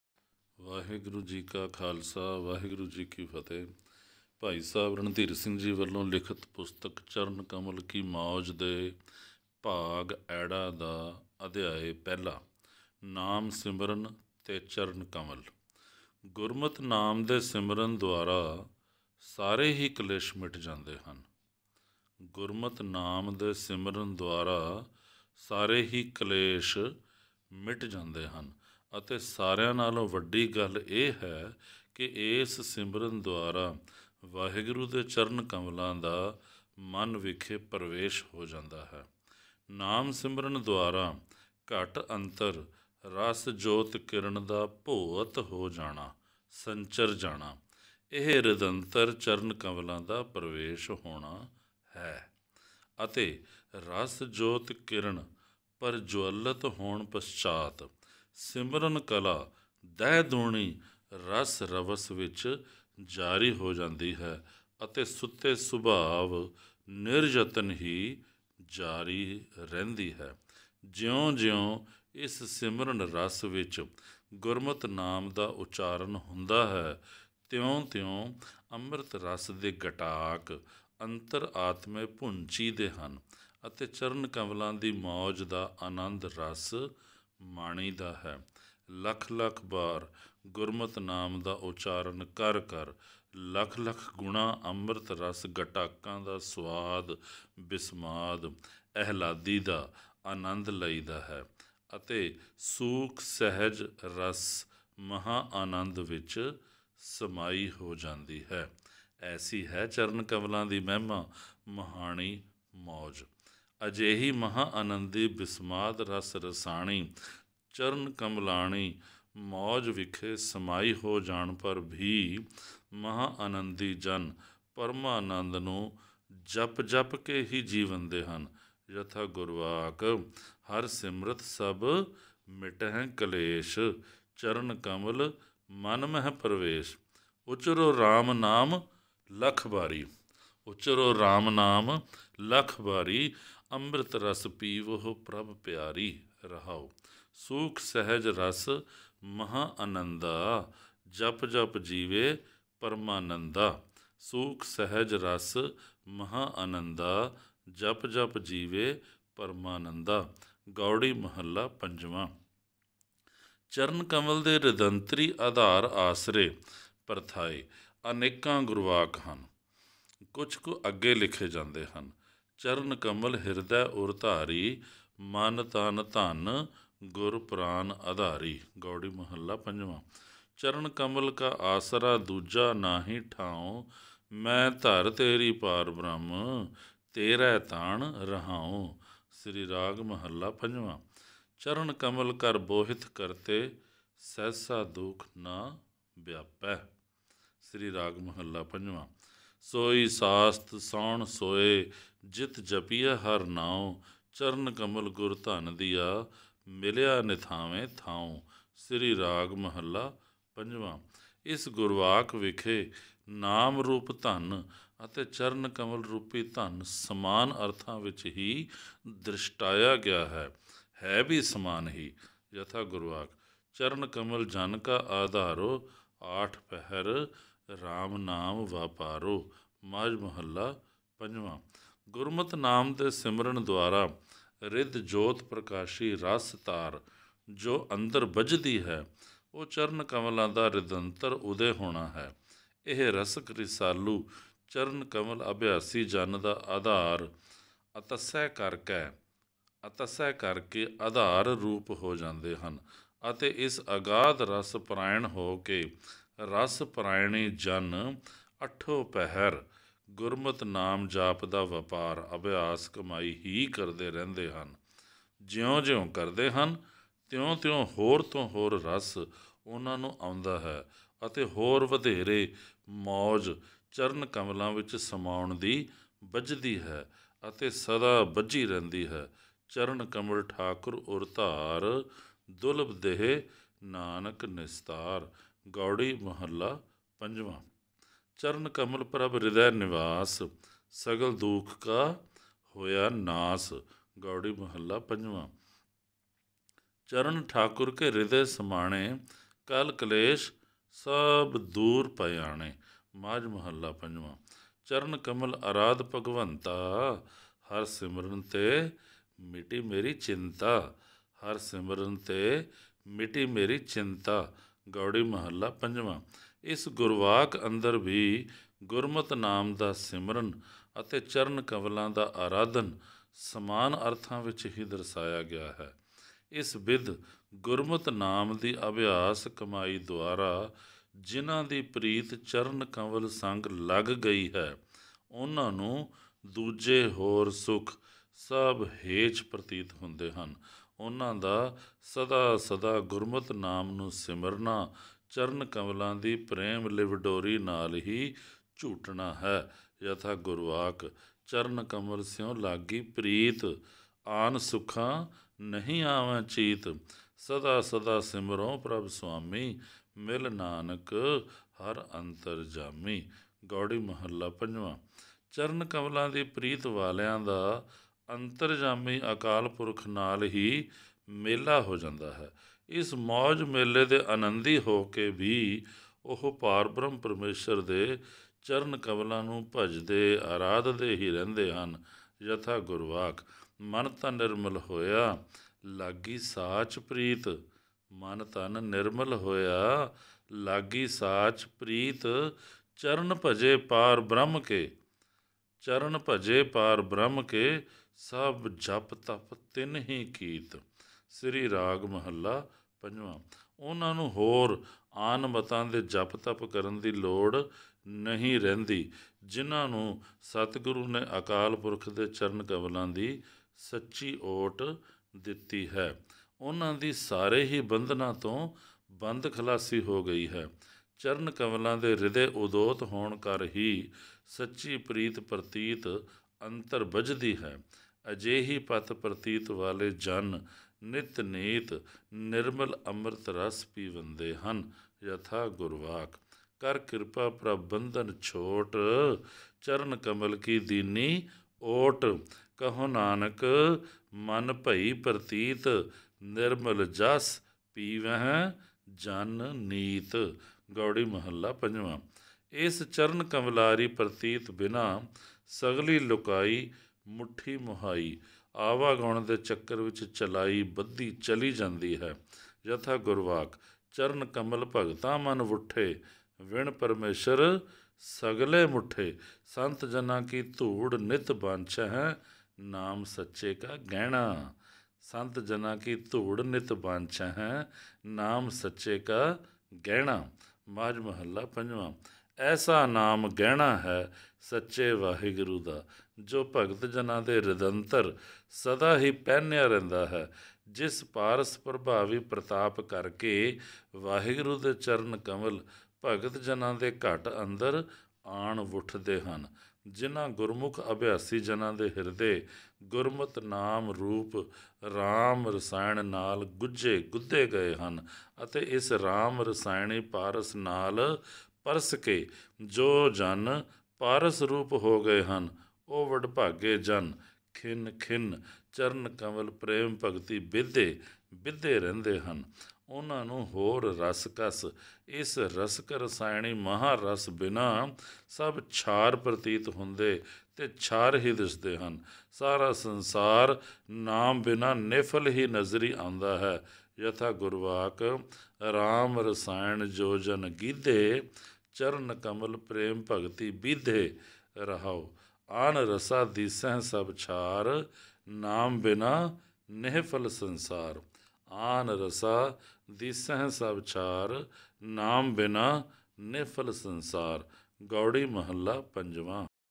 Charan Kamal Ki Mauj, ਚਰਨ ਕਮਲ ਕੀ ਮਉਜ : ਅ - (੧), चरण कमल की मौज
Charan Kamal Ki Mauj, ਚਰਨ ਕਮਲ ਕੀ ਮਉਜ : ਅ - (੧)
ਭਾਈ ਸਾਹਿਬ ਰਣਧੀਰ ਸਿੰਘ ਜੀ ਦੁਆਰਾ ਲਿਖਤ ਪੁਸਤਕ ।
Reader / ਪਾਠਕ : ਗੁਰਜਿੰਦਰ ਪਾਲ ਸਿੰਘ